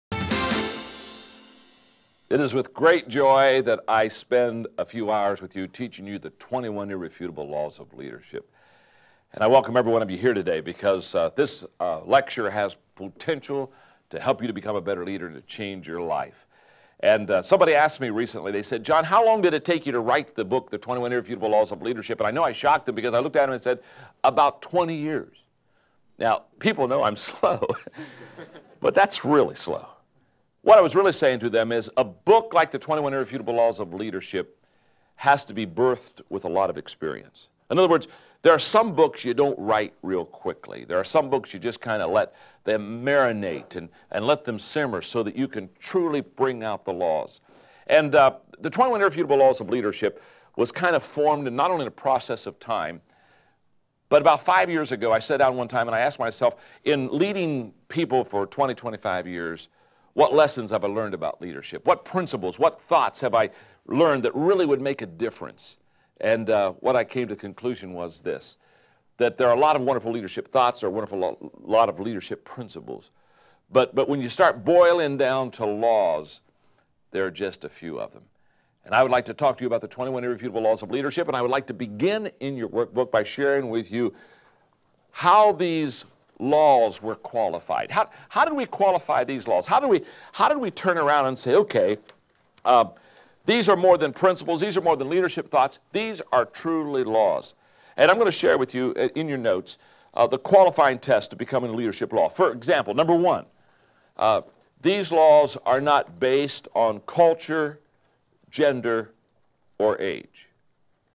Learning the 21 Irrefutable Laws of Leadership
John Maxwell on a short introduction about The 21 Irrefutable Laws of Leadership